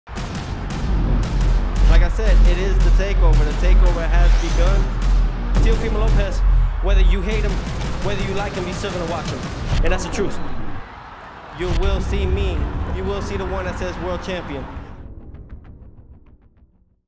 Taking Over The Main Event
On July 19th, ESPN's 2019 'Prospect of the Year' Teofimo Lopez main events his first-ever card LIVE and exclusively on ESPN+ beginning at 10 p.m. ET/7 p.m. PT.